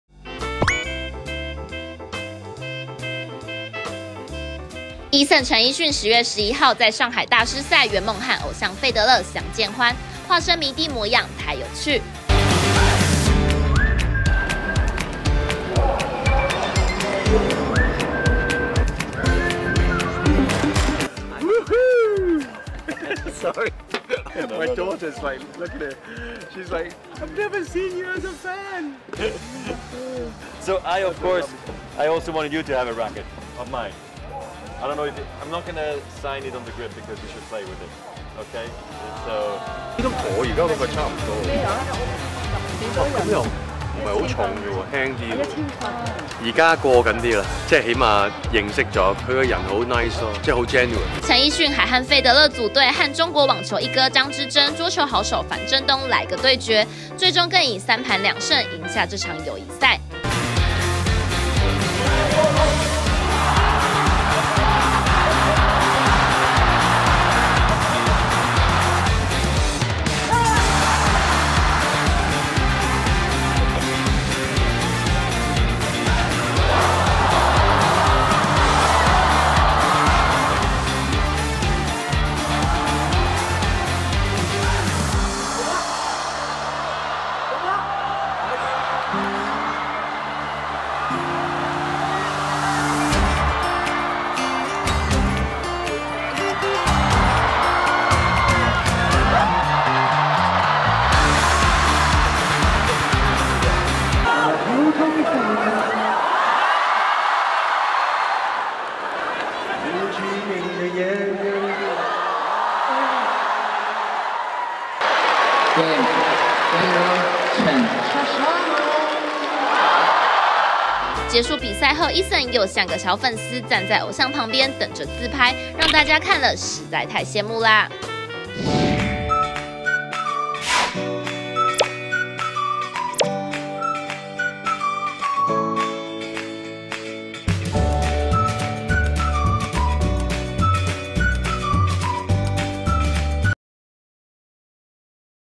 【撩星聞】陳奕迅組隊費德勒對決樊振東 圓夢化身迷弟等自拍模樣太逗
Eason陳奕迅10/11在上海大師賽，圓夢和偶像費德勒相見歡，化身迷弟模樣太有趣。